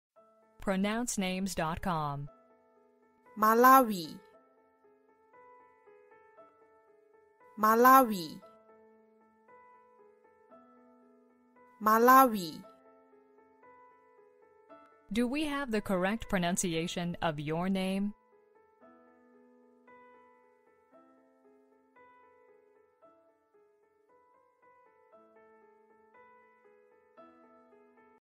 How do you say Malawi in Malaysia? Malaysian pronunciation of Malawi - Pronounce Names
How do you pronounce Malawi in Malaysia? For more information about Malawi, such as gender, origin, etc.,